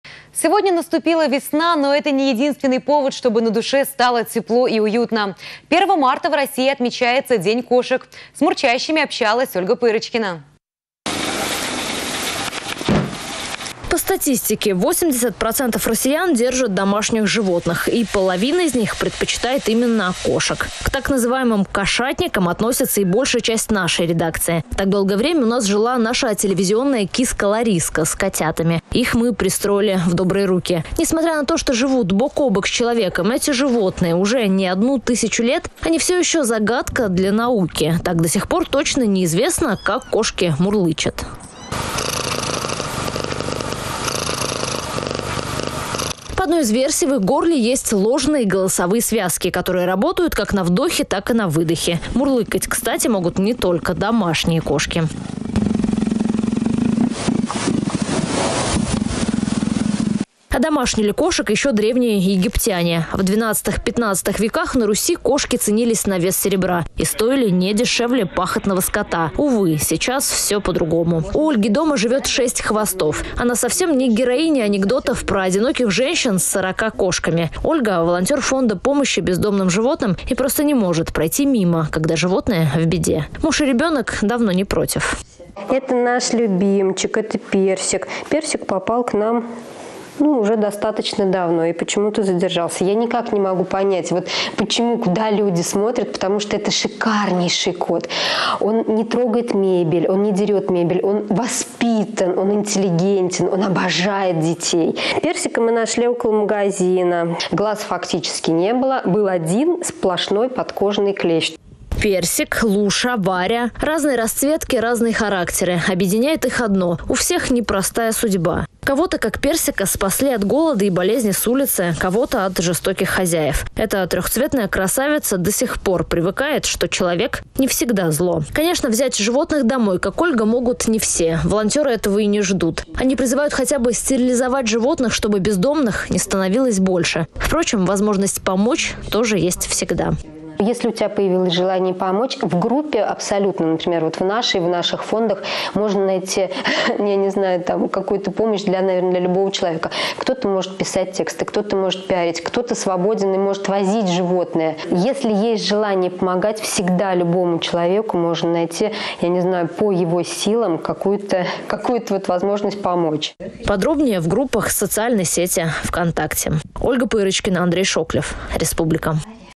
1 марта — Всемирный день кошек
Сегодня наступила весна. Но это не единственный повод, чтобы на душе стало тепло и уютно. 1 марта в России отмечается день кошек. С мурчащими общалась Ольга Пырочкина.